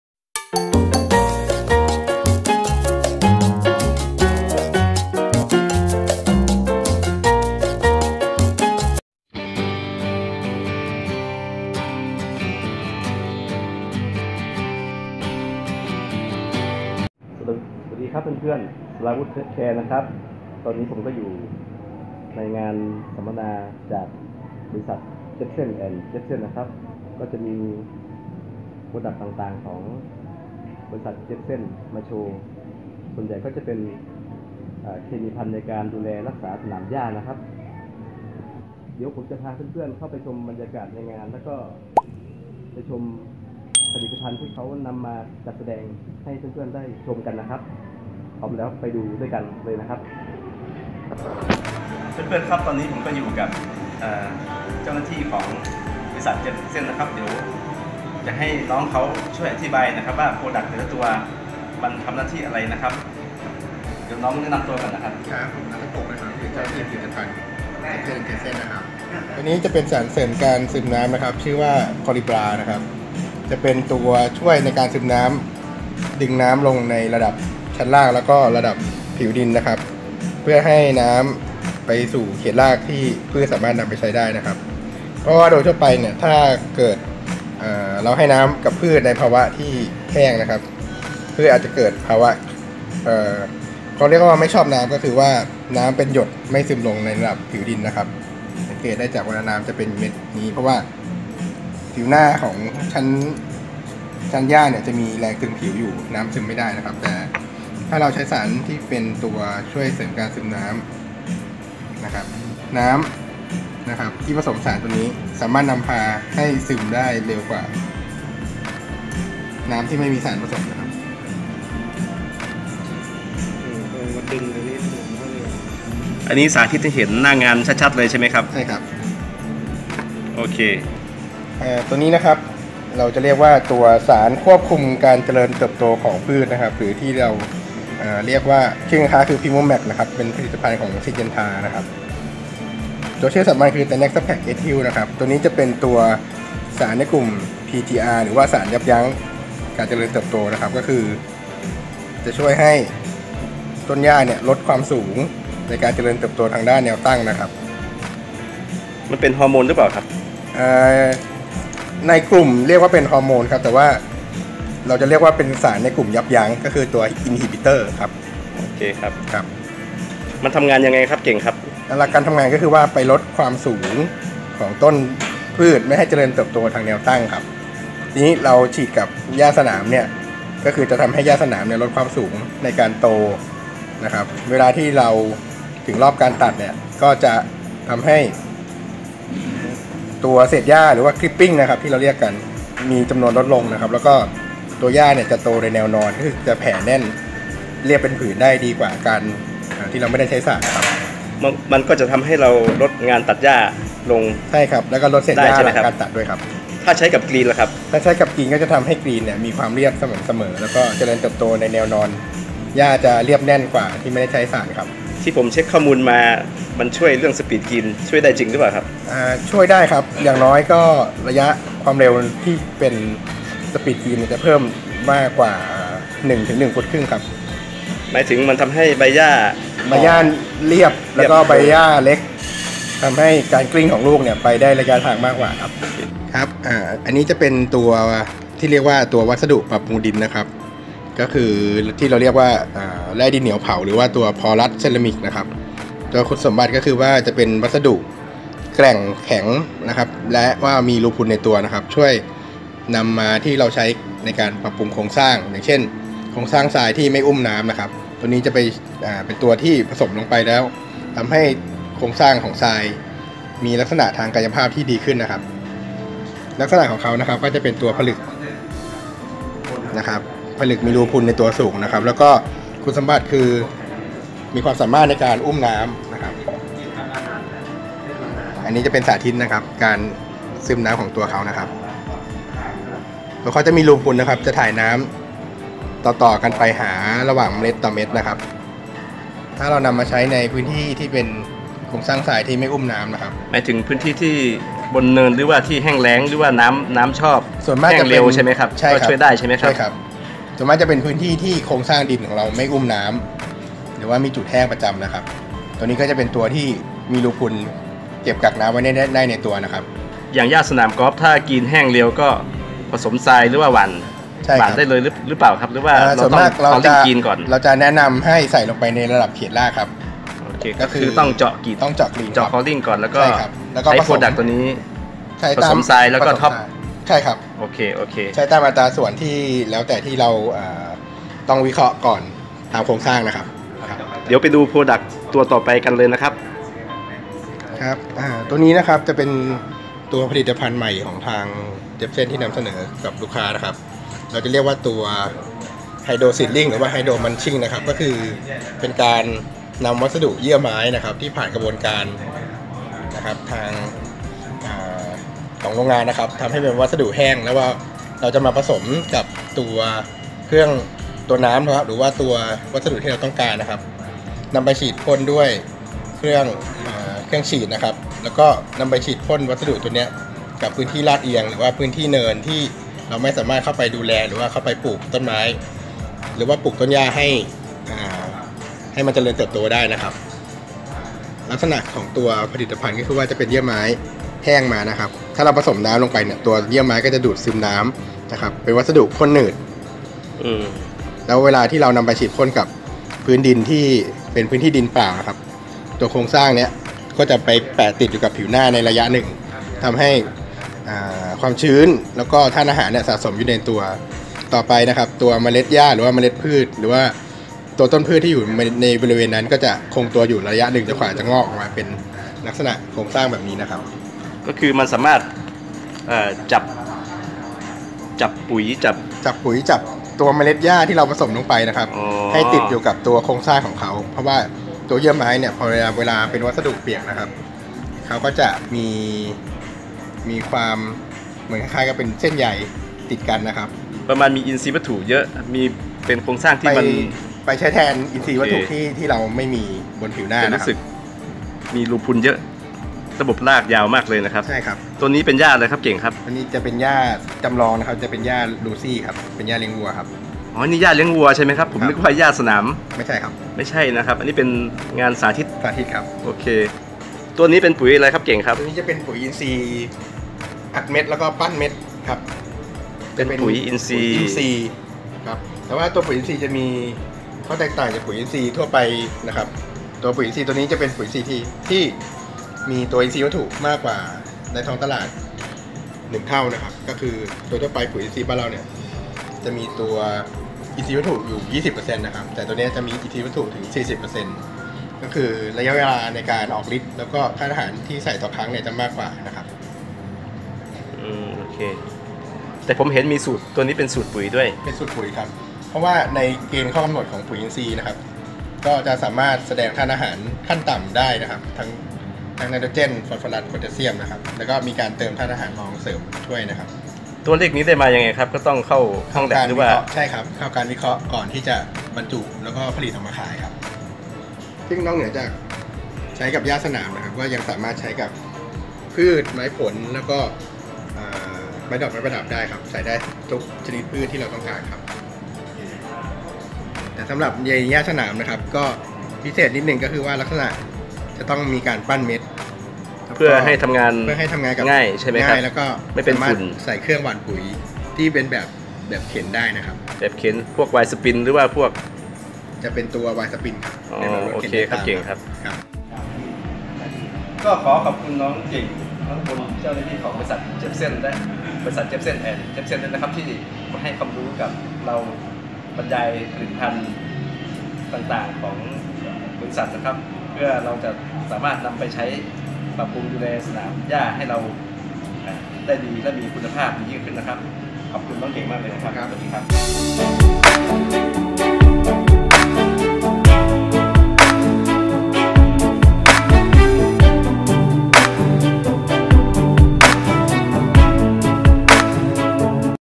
งานสัมมนา บ.เจบเซน แอนด์ เจสเซน เทคโนโลยี หัวหิน ep1. ผลิตภัณฑ์ที่ใช้บำรุงสนามหญ้า
บ.เจบเซน แอนด์ เจสเซน เทคโนโลยี เป็นบริษัทที่จำหน่ายเครื่องจักรดูแลสนามหญ้าและเคมีภัณฑ์ต่างๆ ในประเทศไทย ในงานสัมมนาได้จัดผลิตภัณฑ์บางส่วนมานำแสดงสาธิต เพื่อเป็นแนวทางใหม่ๆในการดูแลรักษาสนามหญ้า

ติดตามทริปดูแลสนามหญ้า